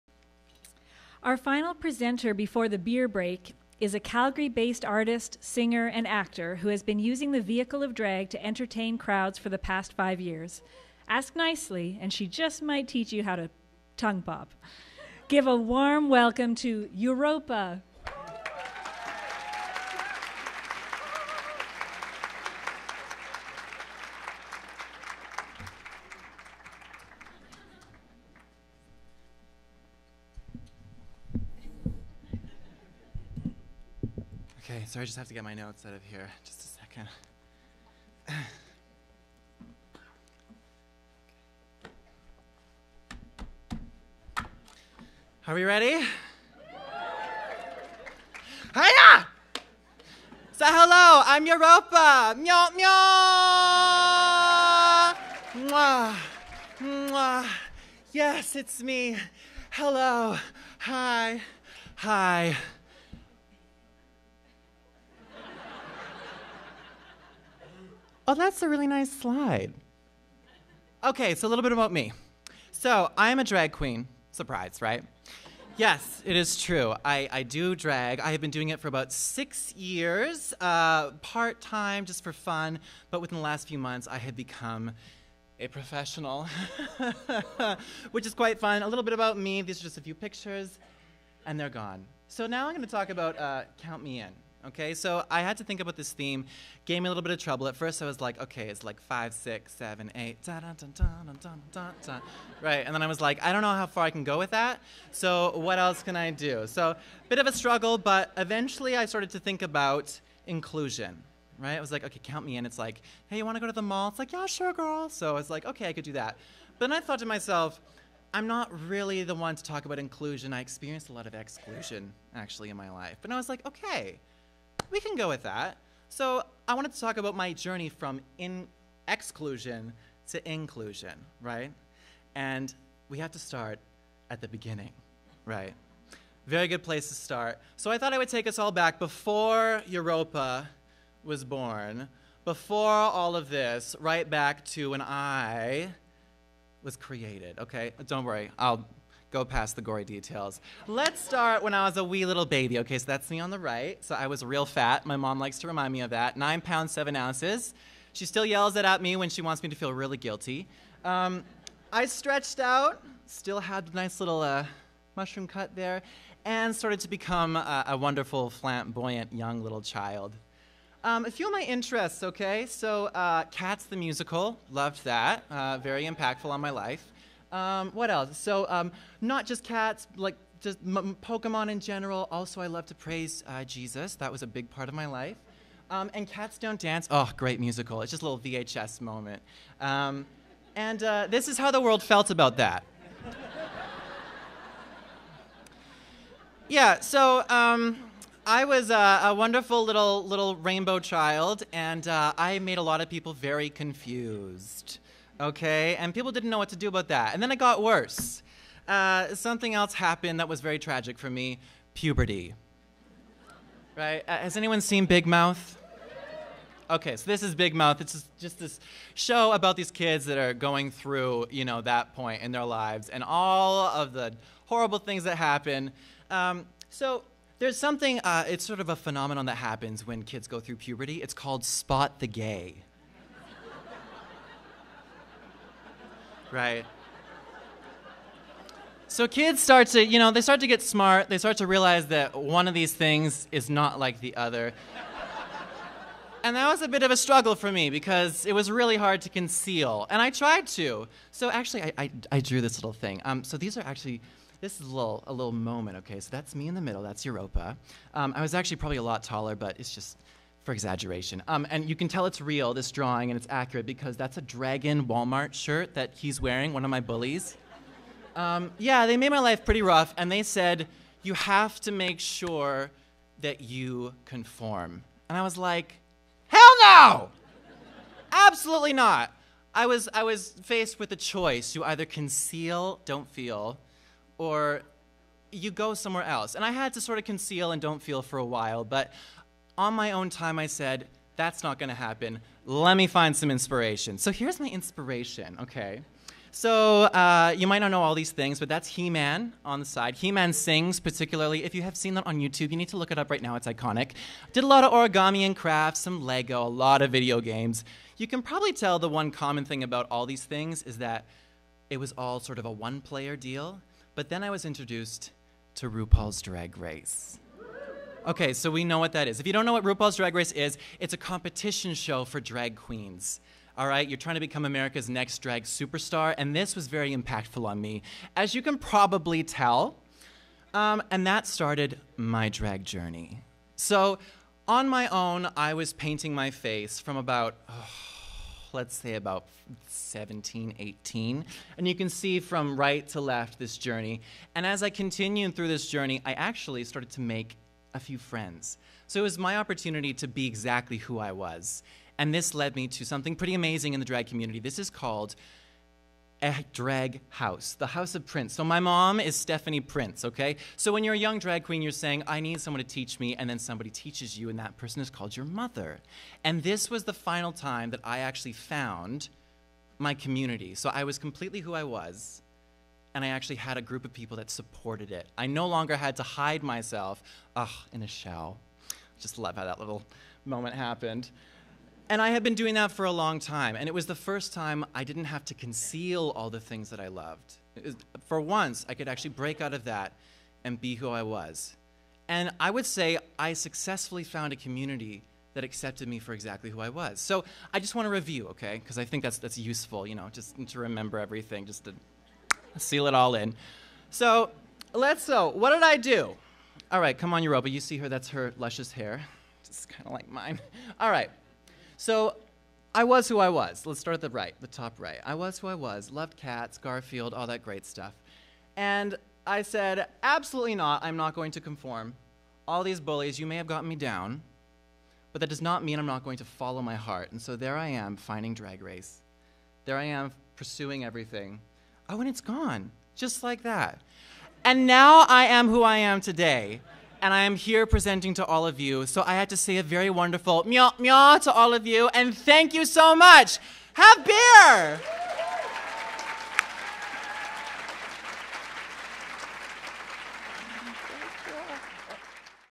Uropa (She/Her) - PechaKucha Calgary #39: COUNT ME IN at Globe
Uropa is a Calgary based artist, singer and actor who has been using the vehicle of drag to entertain crowds for the past five years.
Ask nicely and she might just teach you how to tongue pop.